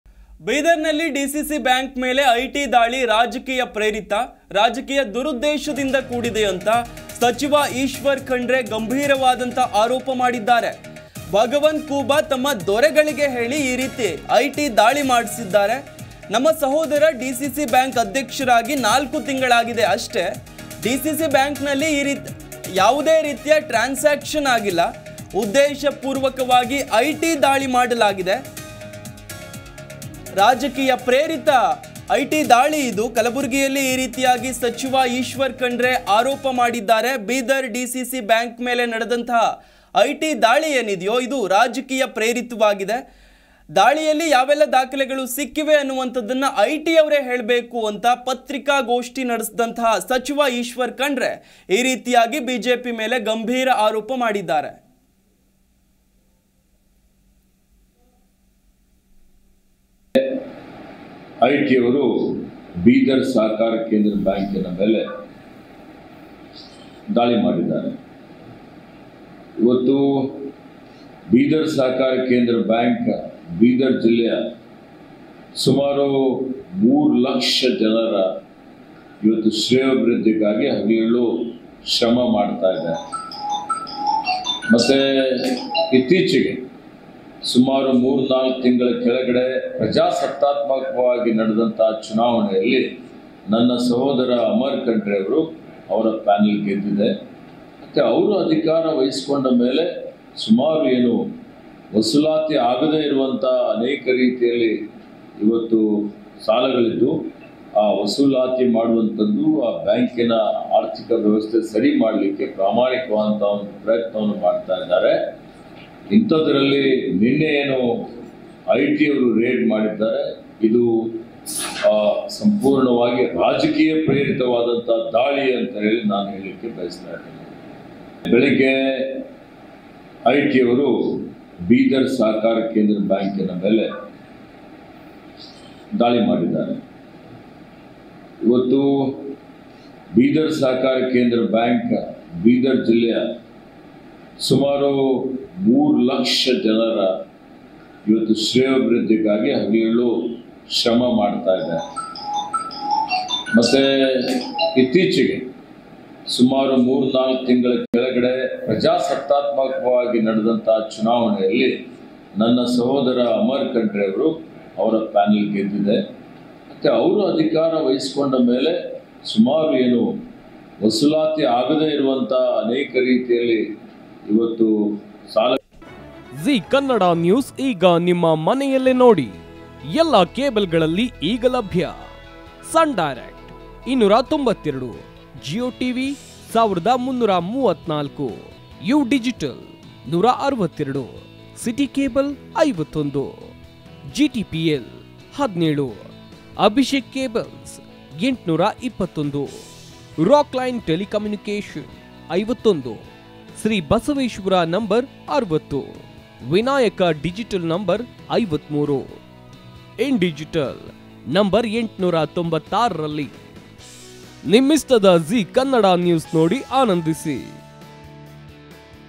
Eshwar Khandre | ED Raid | ಬೀದರ್ ಡಿಸಿಸಿ ಬ್ಯಾಂಕ್ ಮೇಲೆ ಐಟಿ ದಾಳಿ ರಾಜಕೀಯ ಪ್ರೇರಿತ
ಕಲಬುರಗಿಯಲ್ಲಿ ಸಚಿವ ಈಶ್ವರ್‌ ಖಂಡ್ರೆ ಆರೋಪ
ಭಗವಂತ್ ಖೂಬಾ ತಮ್ಮ ದೊರೆಗಳಿಗೆ ಹೇಳಿ ಮಾಡಿಸಿದ್ದಾರೆ
ನಮ್ಮ ಸಹೋದರ DCC ಬ್ಯಾಂಕ್ ಅಧ್ಯಕ್ಷರಾಗಿ 4 ತಿಂಗಳು ಆಗಿದೆ
ಡಿಸಿಸಿ ಬ್ಯಾಂಕ್‌ನಲ್ಲಿ ಯಾವುದೇ ಟ್ರಾನ್ಸಾಕ್ಷನ್ ಆಗಿಲ್ಲ